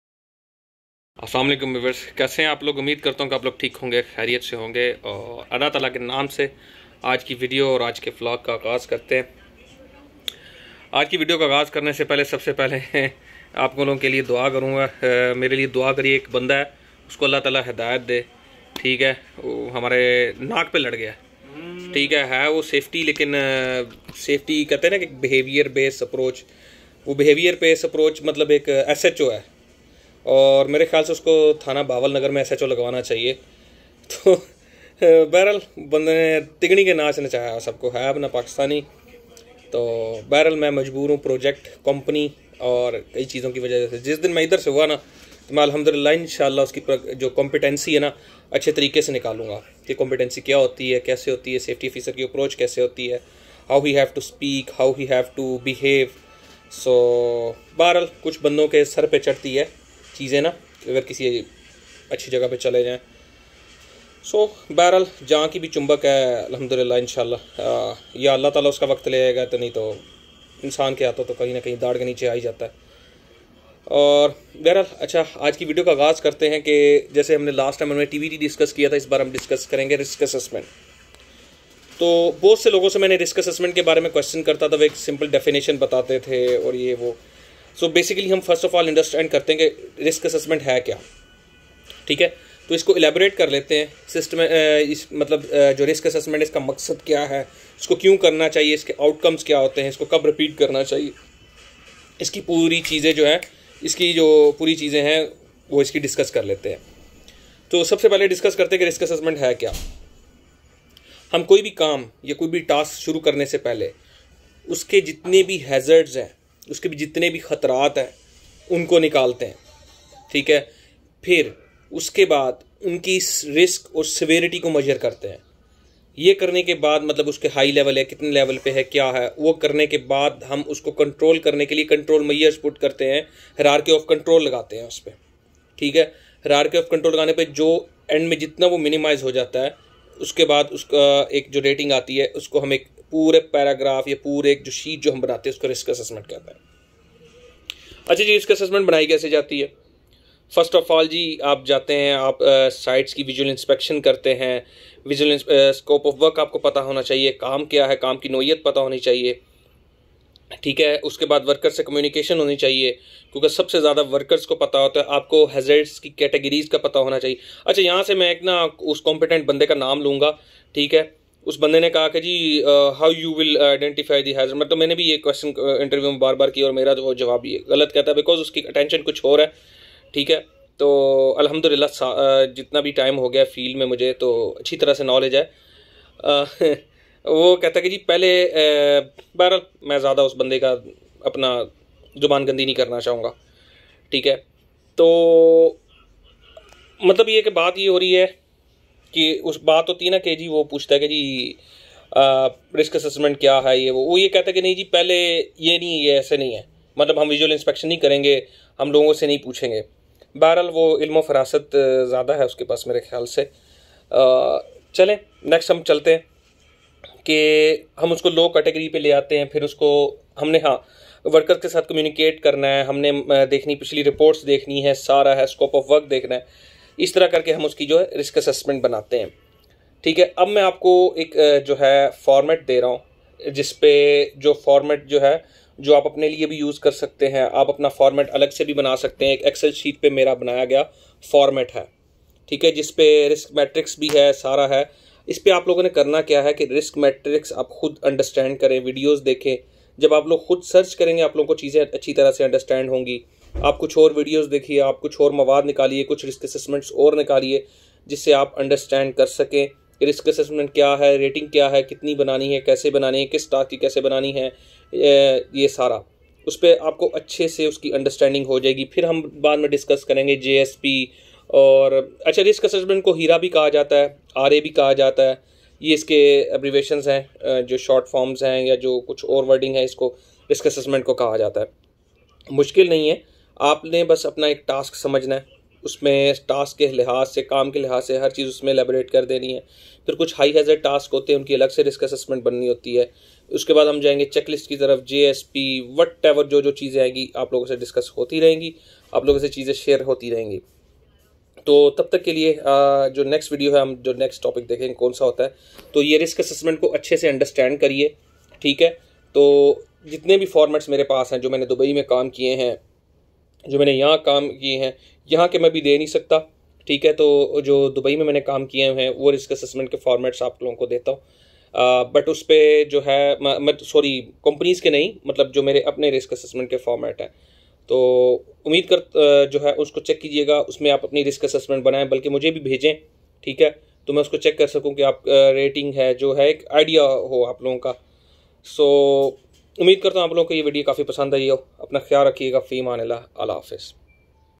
How to make Risk Assessment | What is Risk Assessment
In this video, I am going to share how to make a Risk Assessment and share it's format.